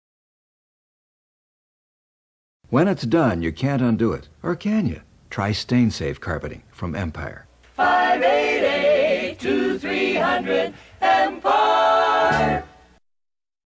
Another Classic Empire Today Commerical
Classic Flashback commercial from Empire Today®! When life gives you stains, Empire® has your covered ⭐️